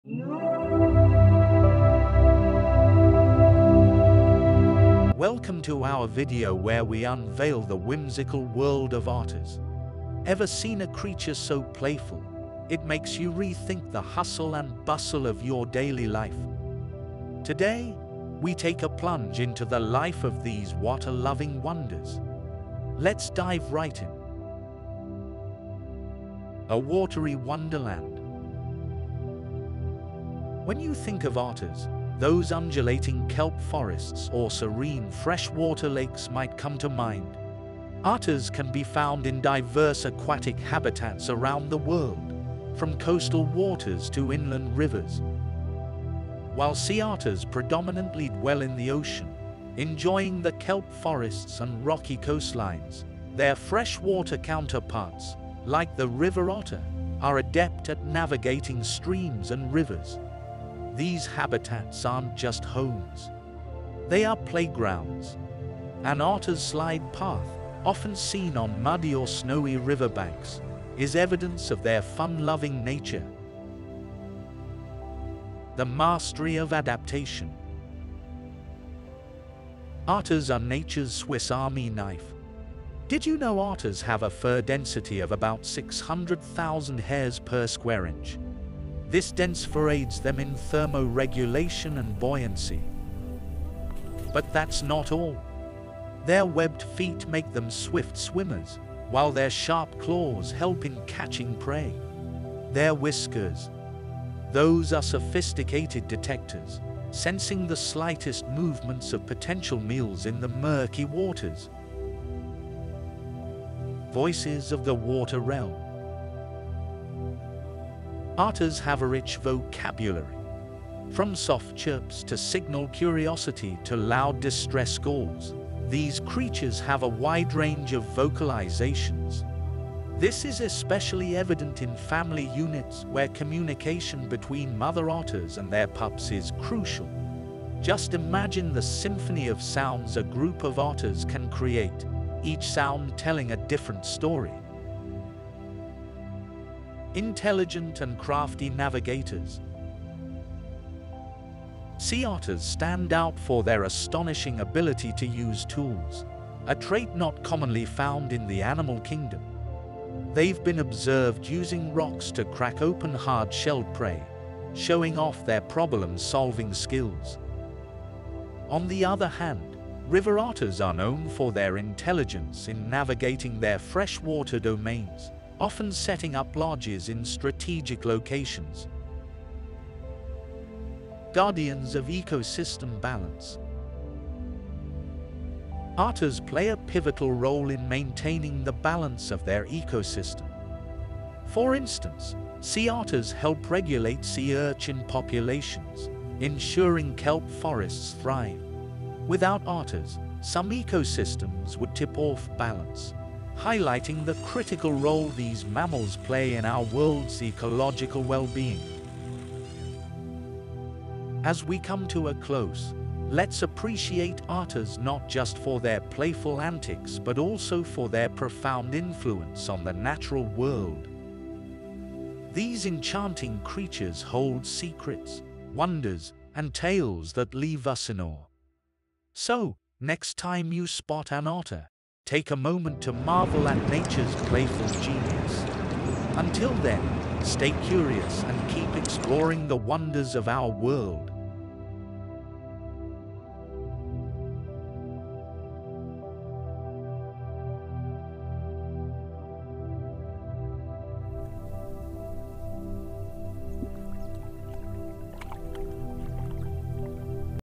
Otters Unveiled: Nature’s Playful Geniuses
Dive into the enchanting world of otters and uncover the many facets that make these water-loving mammals so captivating. From their intriguing social behaviors to their jaw-dropping skills, otters have more than just cuteness up their sleeves. Journey with us to explore lesser-known details, fun trivia, and surprising facts about these wonderful creatures. By the end, you'll have a newfound appreciation for these delightful denizens of the deep!